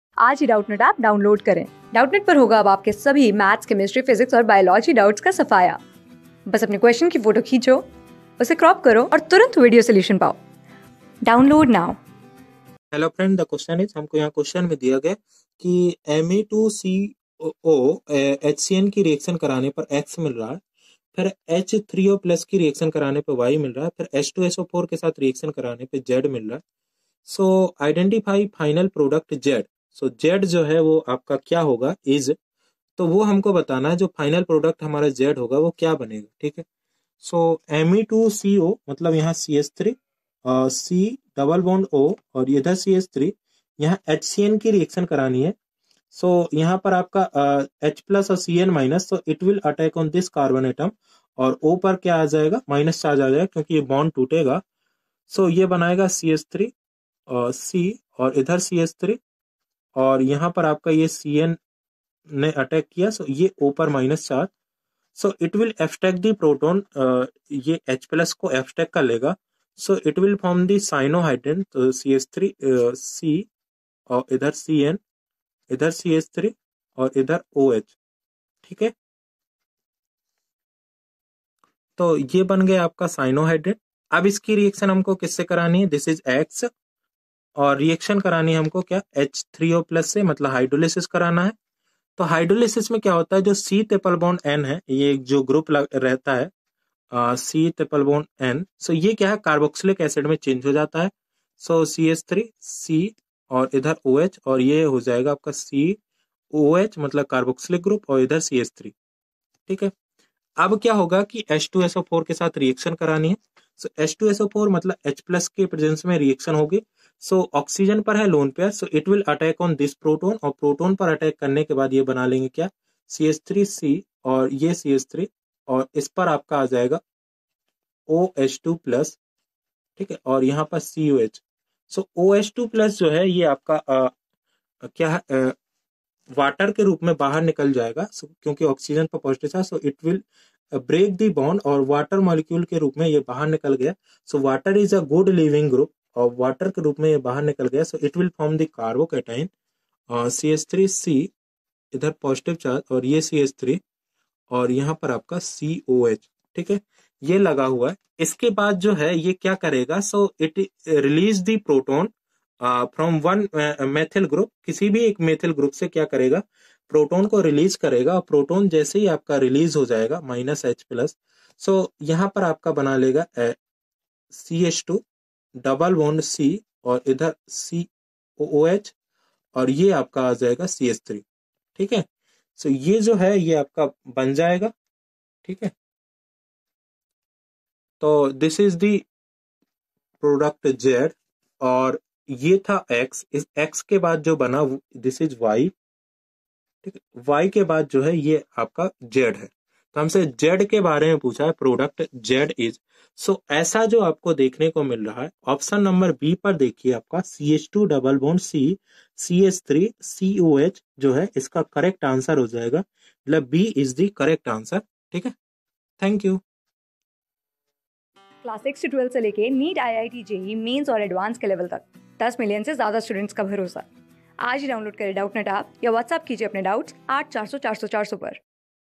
Identify the final product (Z) in the following sequence of reactions : Me_2C=O+HCNrarr(X) overs...
Identify the final product (Z) in the following sequence of reactions : Me_2C=O+HCNrarr(X) overset(H_3O^+)rarr(Y)overset(H_2SO_4)rarr
Class: 12
Subject: CHEMISTRY
Chapter: NTA NEET SET 87
Board:NEET

👉 Have Any Query? Ask Us.